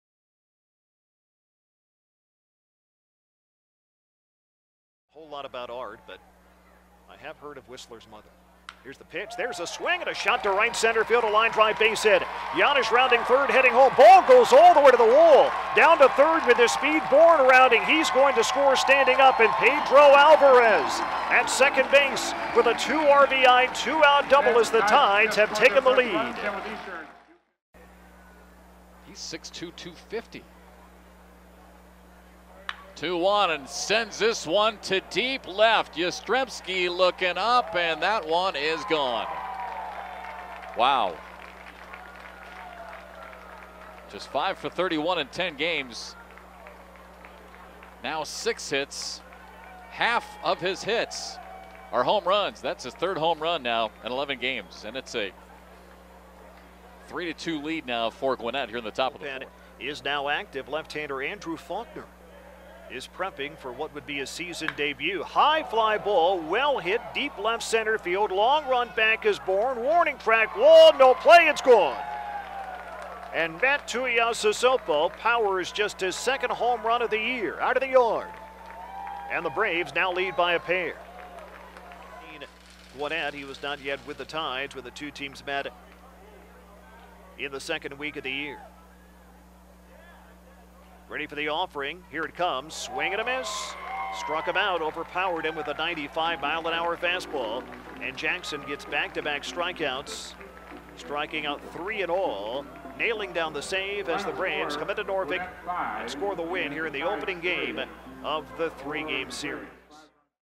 Norfolk Tides vs. Gwinnett Braves -- May 15th, 2017
The Tides come up short in the series opener against Gwinnett, as the Braves win 4-3. Pedro Alvarez lead the Norfolk Tides with a 2-run double.